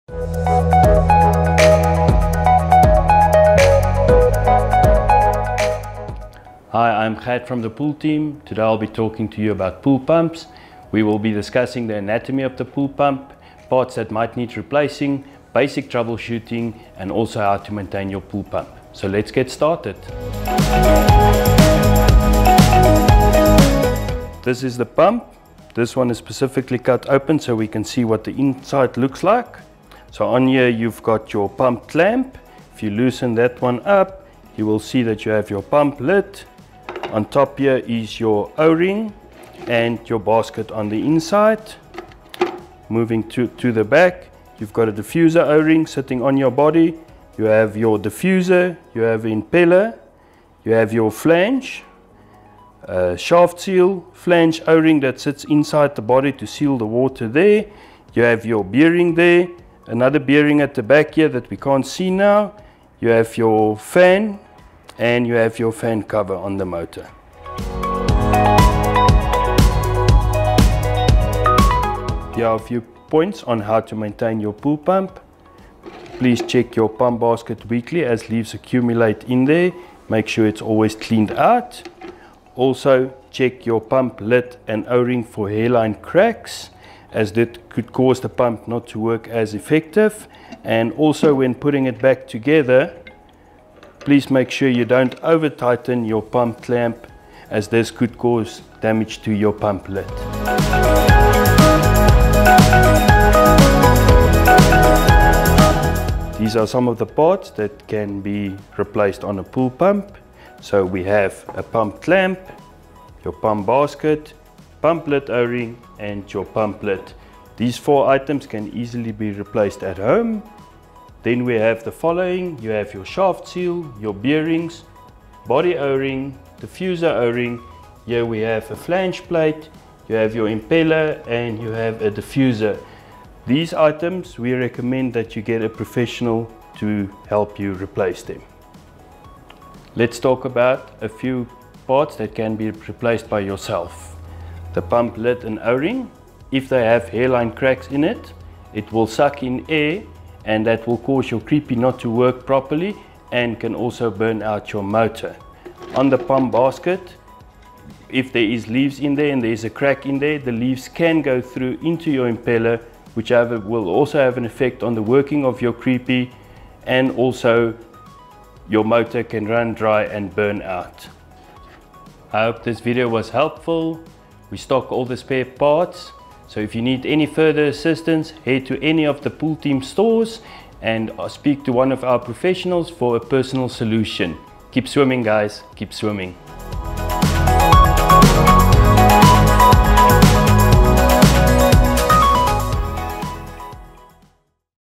Swimming Pool Pumps DIY | How to replace parts
Learn all you need to know about a Pool Pump with The Pool Team!
The pool pump is an essential part of the swimming pool system. The pump draws in water from the pool and sends it through the filter, which is where debris gets trapped before returning the water back to the pool.
Watch this video to learn about the anatomy of a pool pump, how to maintain your pump, troubleshooting and replacing parts on a pool pump.

Jump quickly to these moments:

0:00 Introduction
0:23 Anatomy of a pool pump
1:10 How to maintain a pool pump
1:45 Replacing Part and Trouble Shooting